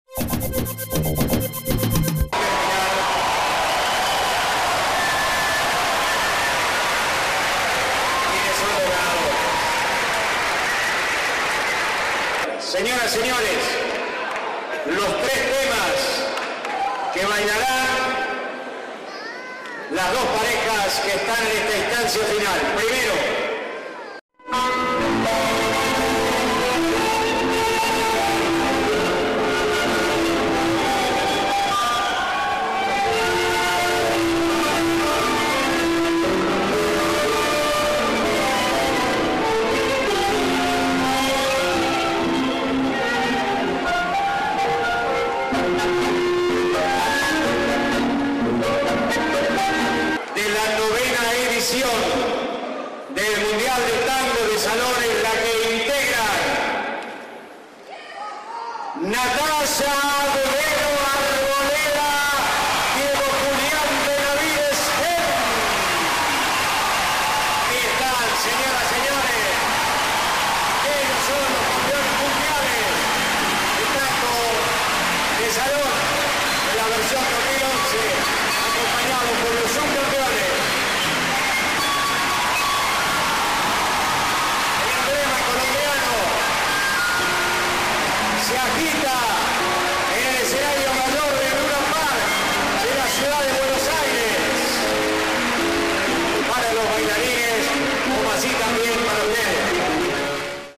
Des Colombiens remportent la coupe du monde de tango
Un couple colombien a remporté les championnats du monde de tango, dans la catégorie "tango de salon" à Buenos Aires. Durée: 01:40